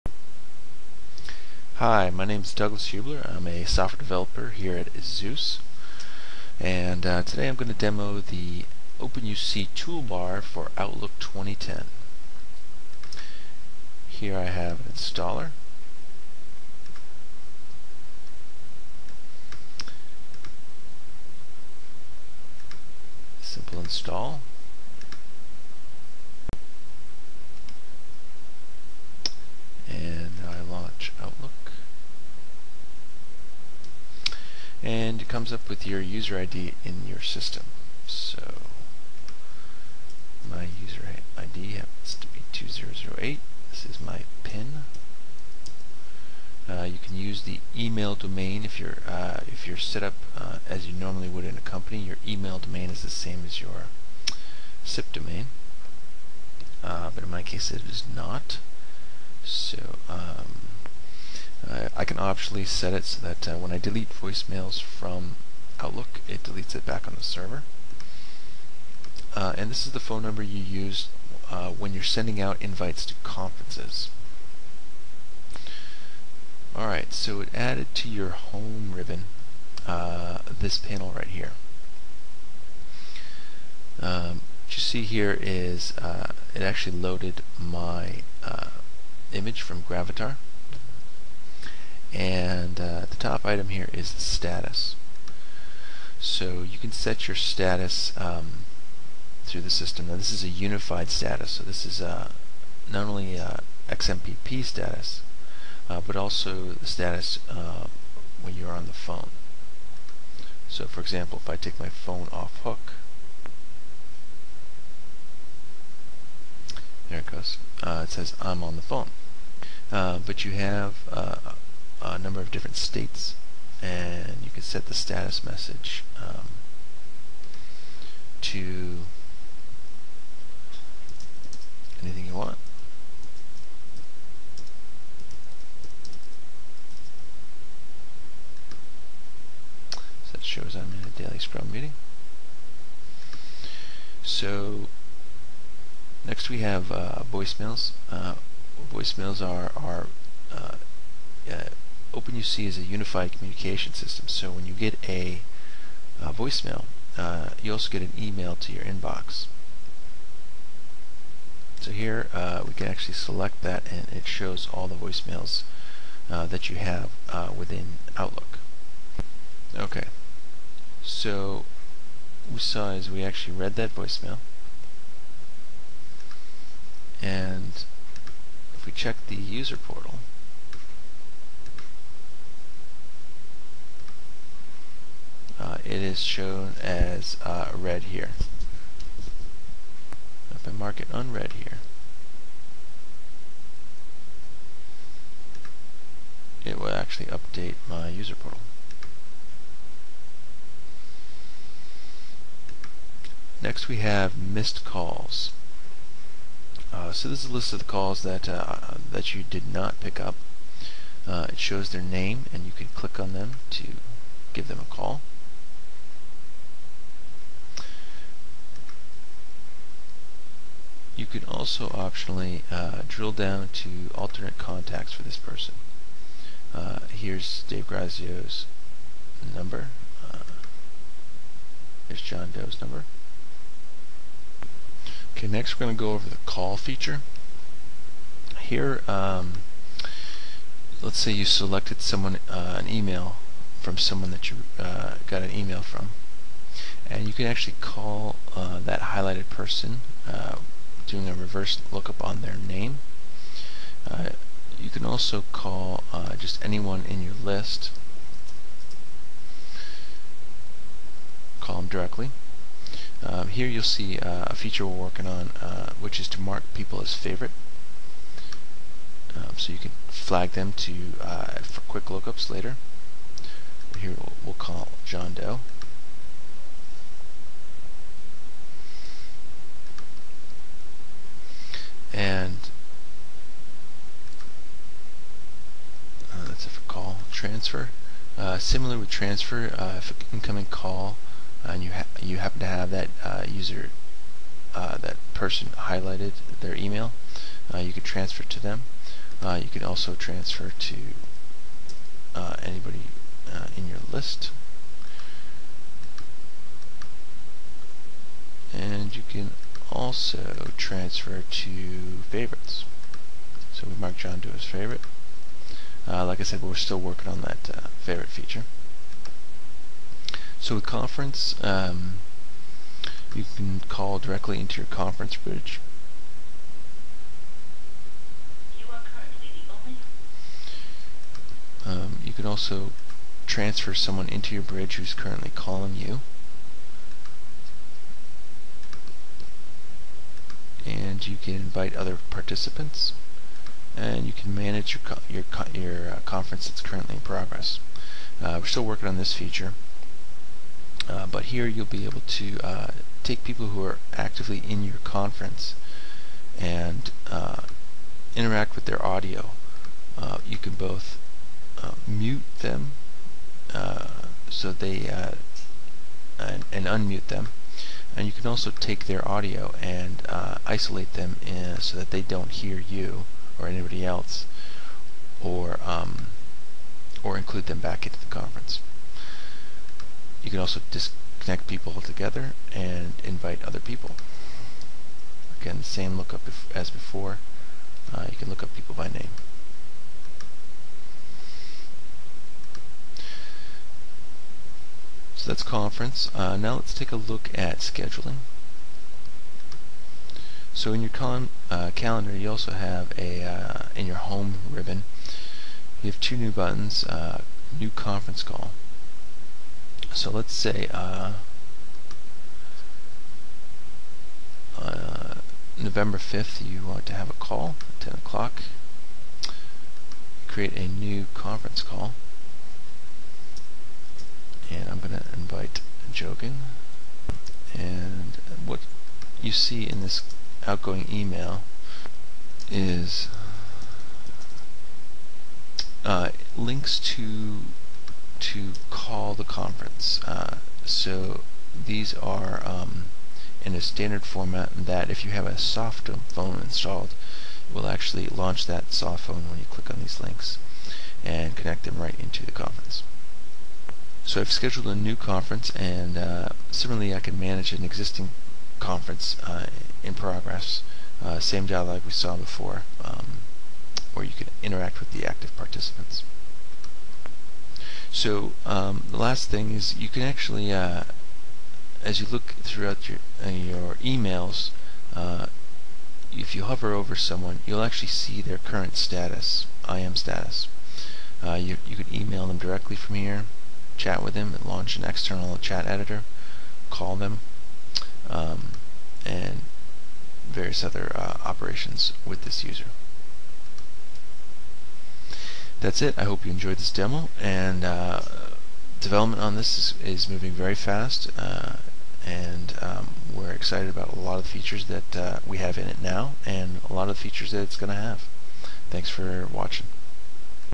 openuc-toolbar.mp4
Highlight the eZuce openUC integration capabilities with Microsoft Outlook